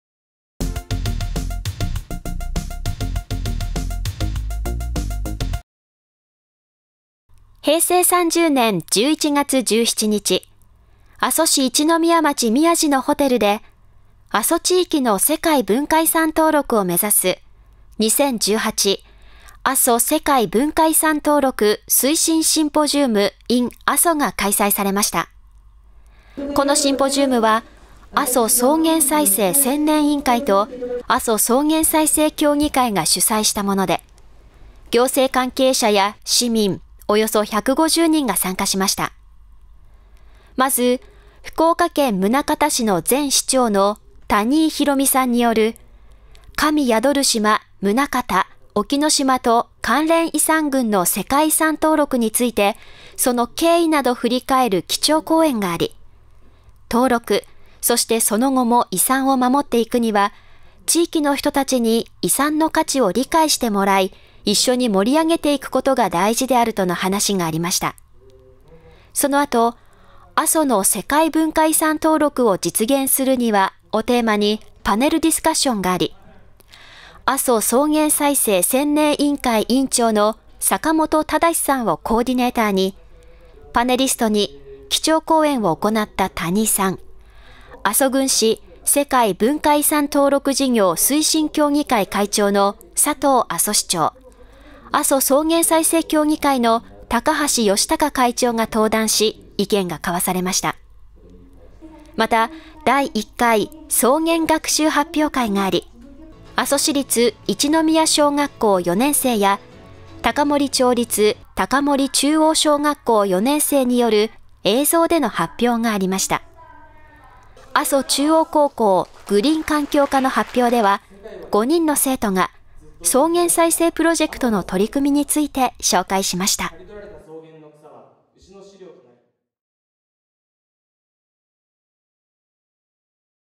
2018阿蘇世界文化遺産登録推進シンポジウムin阿蘇
阿蘇地域の世界文化遺産登録を目指す2018阿蘇世界文化遺産登録推進シンポジウムin阿蘇が開催されました。